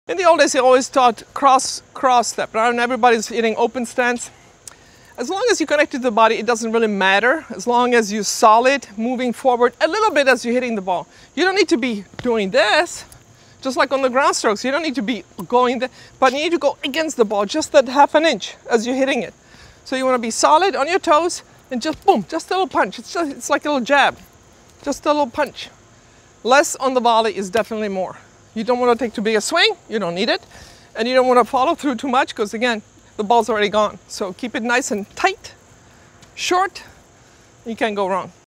Martina Navratilova | Proper Volley Technique | TopCourt
An effective volley isn’t much use without proper technique and skills at the net. Martina Navratilova teaches you the proper technique to cover the net and always be in position to finish the point.